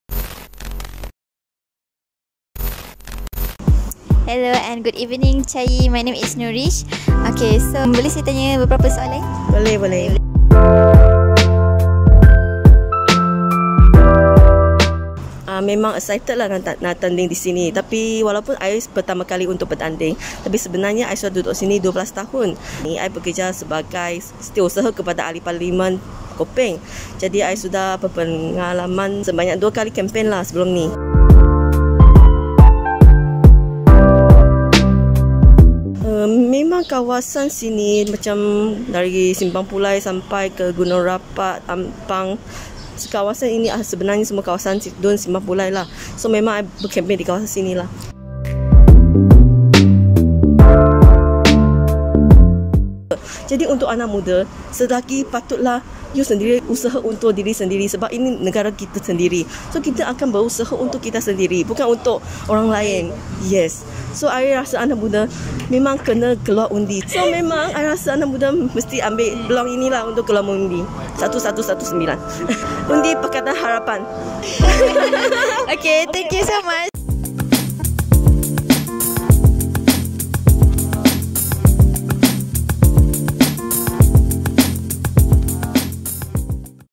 Wong Chai Yi mengambil alih Simpang Pulai
Wong Chai Yi, calon yang mewakili PKR di Simpang Pulai, kerusi DUN N45 pada pru15 kali ini. Untuk pengetahuan korang kan dulu beliau merupakan bekas setiausaha ahli parlimen Gopeng tau sekarang ambil alih di Simpang Pulai pula. Hebat sungguh beliau. Boleh jadikan insprasi untuk orang-orang muda.